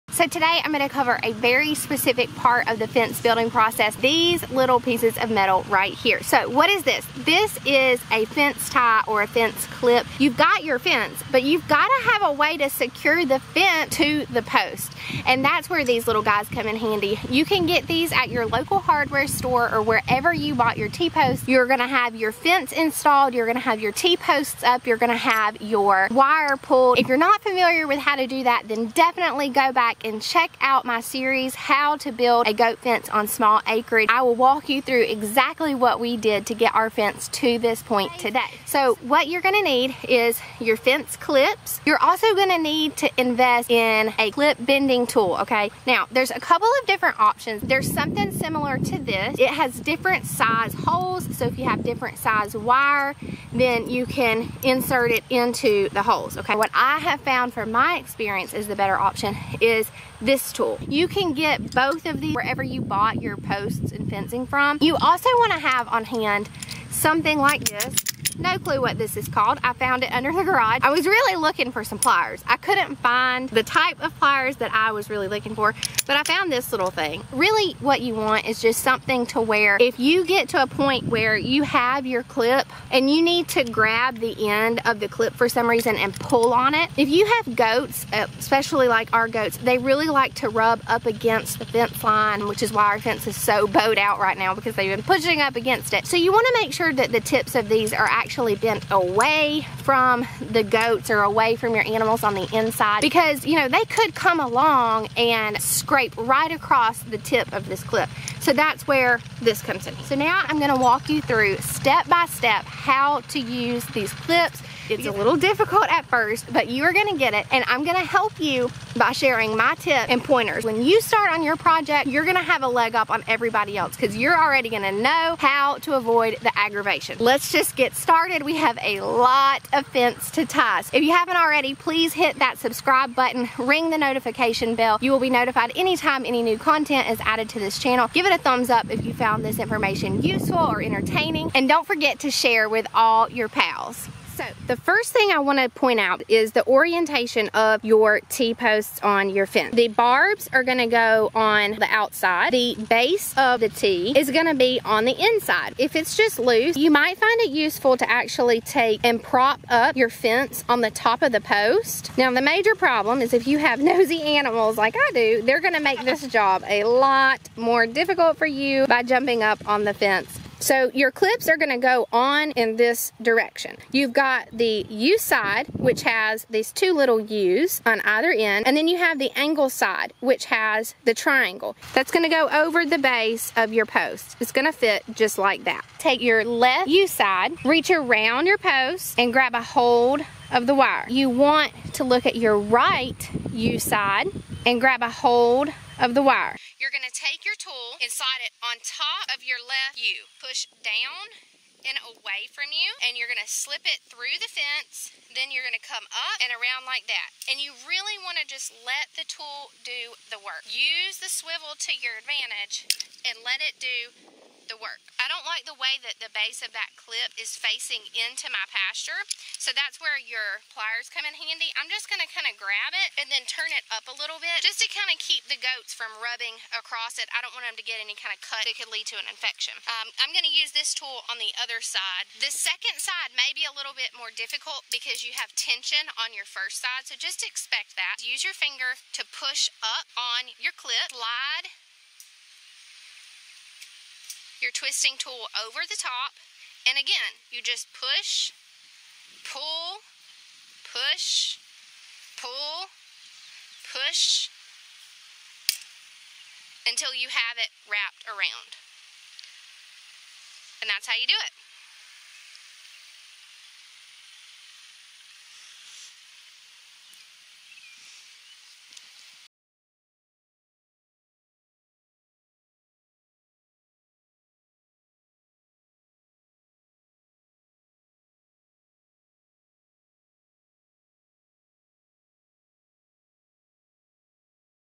How To Apply T-Post Clips I THE EASY WAY
Welcome to Meaningful Life Homestead!
     The most difficult part of our fence building process has been applying fence ties/clips to the wire fencing on our T-Posts. If it is your first time tying up wire fencing, you could be in for a real headache. Thankfully for you, I am sharing my tips and tricks in the episode in hopes of saving your the trouble. It is so important to secure your fence to the T-Posts properly in order to keep your livestock safe. I have found that by using the right clip bending tool and adopting a step by step application process, tying fence can actual be a breeze!

Check out the other videos in this series "How We Built a Fence on Small Acreage" to see what steps we took to install our fence.

I would appreciate your feedback in the comments below!

Love,
Christina